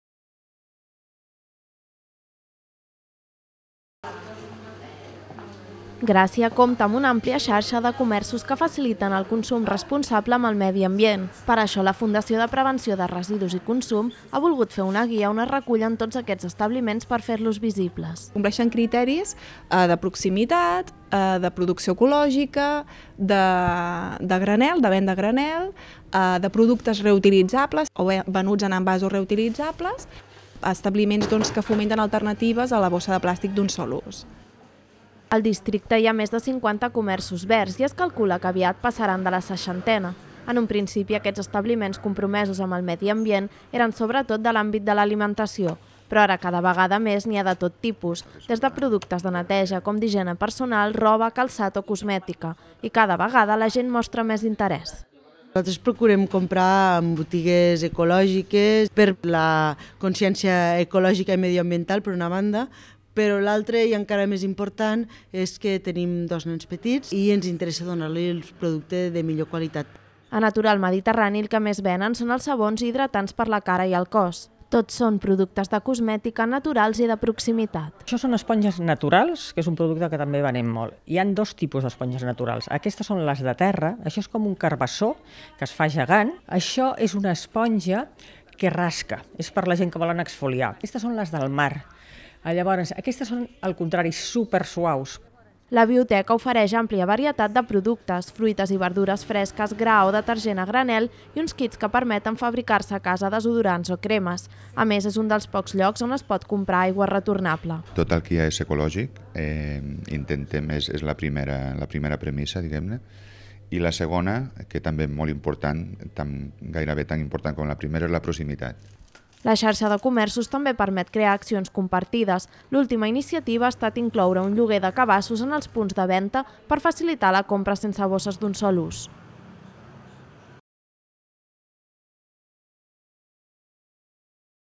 Gràcia, el barri amb més comerços sostenibles
Gràcia és un dels districtes de la ciutat de Barcelona on es concentren més establiments que faciliten al consumidor la compra de productes respectuosos amb l’entorn. És per això que l’any 2013 la Fundació de Prevenció de Residus i Consum Responsable va veure oportú crear una guia de bones pràctiques en què sortissin tots aquests comerços verds. La idea és que els ciutadans coneguin tot el que hi ha al seu abast per poder fer un consum responsable. Més informació